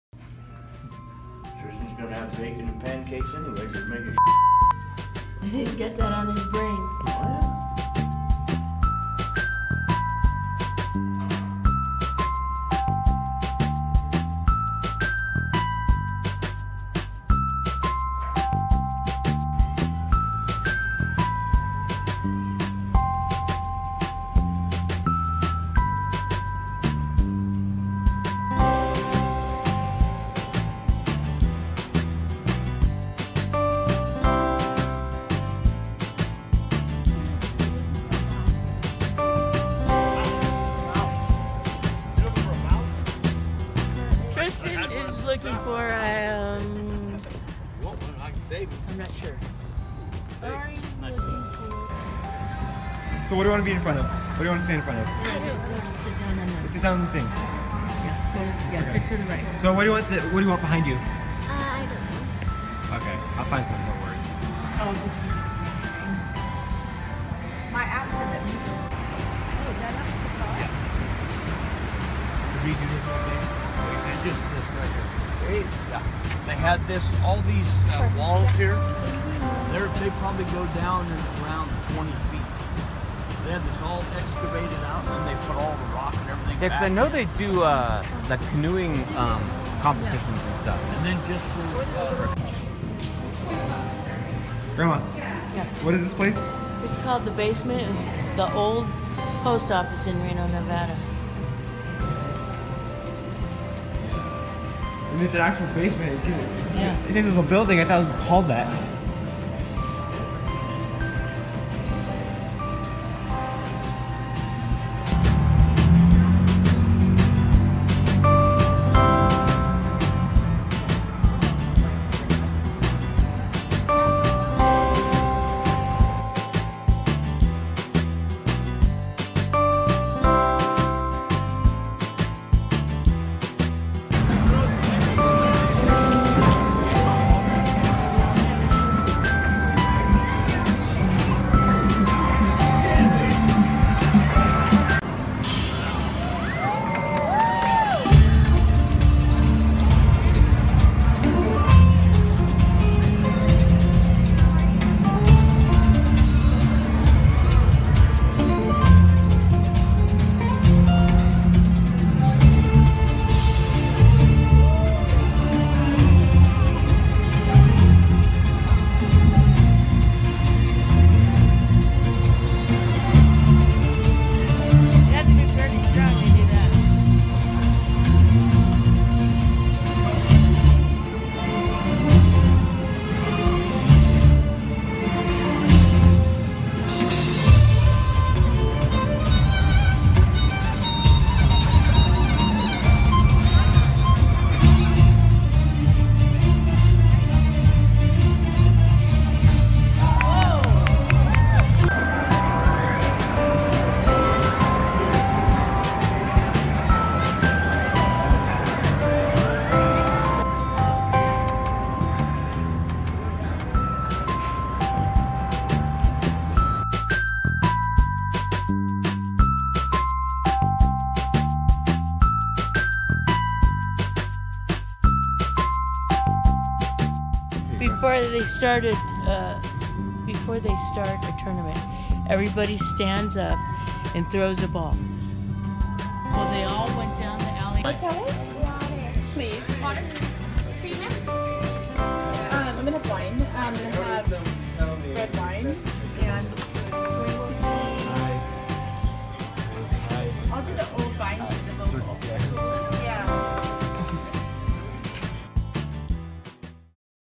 Fun Day in Reno
Spent a really fun day with my family in Reno, tomorrow, we will be going farther up Nevada to Winnemucca.